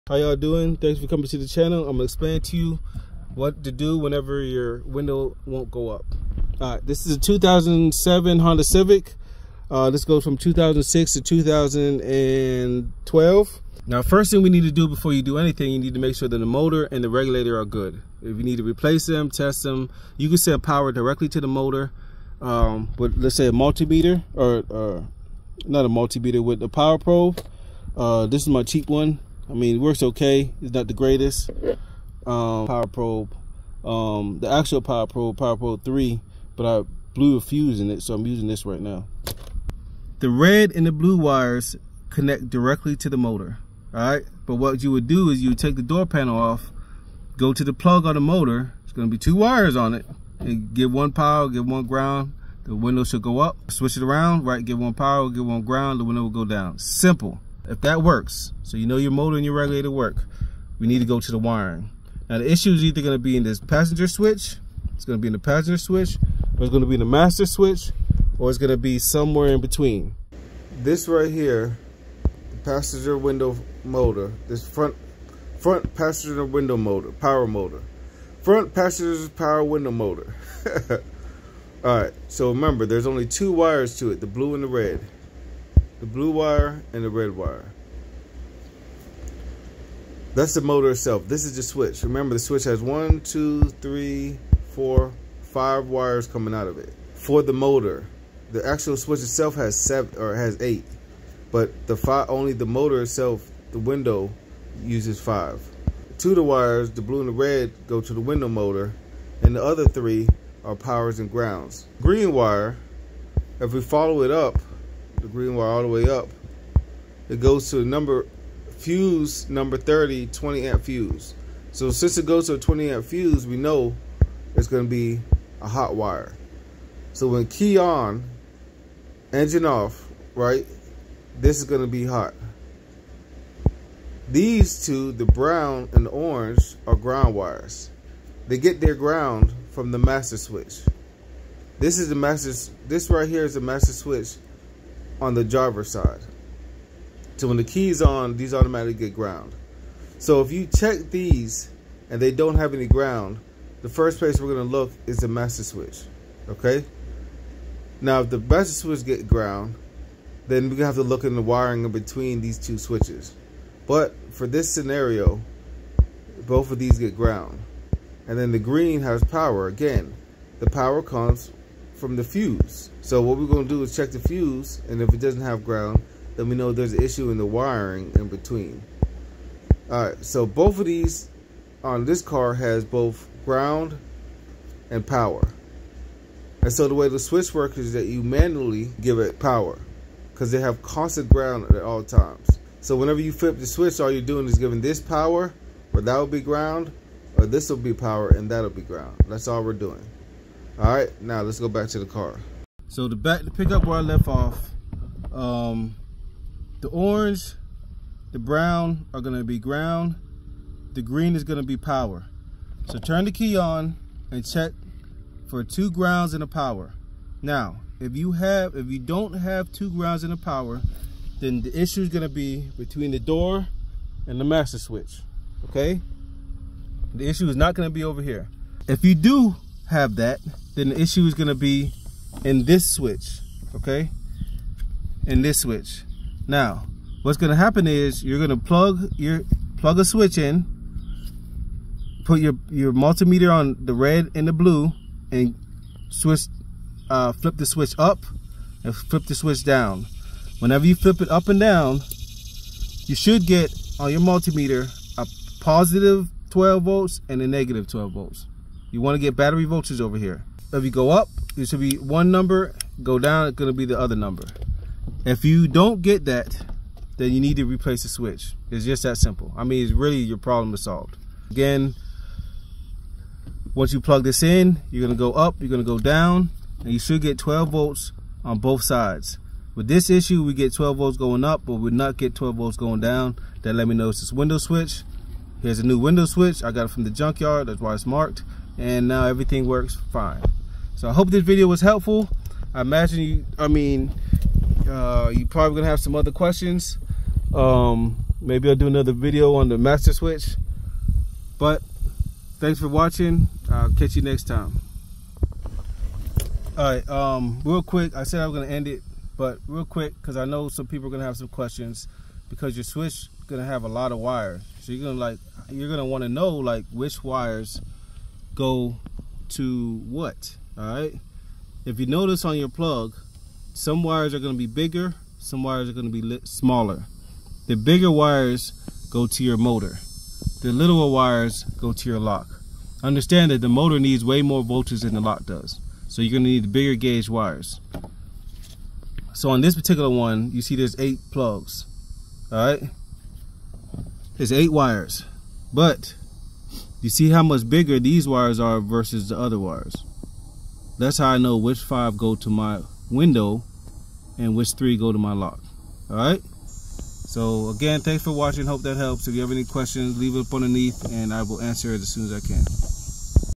honda civic window wiring diagnosis
this video is to explain how to check and possibly diagnose wiring issues you might be having with your vehicles window. the vehicle used is an 07 Honda civic 1.8l but the basic principles apply to many many vehicles